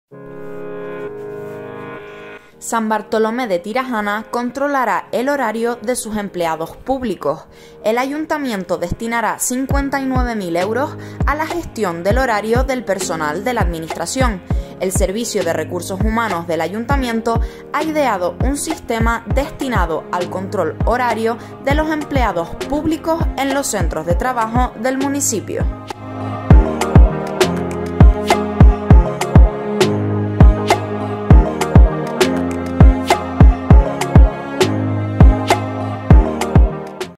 San Bartolomé de Tirajana controlará el horario de sus empleados públicos .
El Ayuntamiento destinará 59.000 euros a la gestión del horario del personal de la Administración
El Servicio de Recursos de Humanos del Ayuntamiento de San Bartolomé de Tirajana, liderado por la Alcaldesa , Conchi Narváez , ha implantado un sistema destinado al control horario de los empleados públicos en los centros de trabajos del municipio. Este sistema está basado en la lectura de huellas dactilares, mediante terminales u otros sistemas de identificación biométricos, de modo que se asocia un código personal a cada usuario y permite cumplir con un imperativo legal.
Durante la anterior legislatura no se puso en marcha ninguna iniciativa que tuviese como objetivo la optimización de la gestión horaria del personal de la Administración Pública del municipio. Esto significa que no se registraba, ni quedaba constancia de las entradas y salidas, gestión de jornadas, solicitudes, permisos y horarios de los empleados públicos.
Por ese motivo, el actual grupo de gobierno ha desarrollado un contrato, con un plazo de ejecución de dos años, prorrogables por dos años más. Además, destinarán una partida presupuestaria de 59.000 euros en total. Con esta iniciativa se busca, no solo, aportar más transparencia, sino también cumplir con lo establecido en la norma legislativa.
Los criterios de adjudicación del contrato tienen en cuenta la mejor relación calidad-precio, basándose en dos criterios principales: la valoración económica y el margen de variación de plantilla.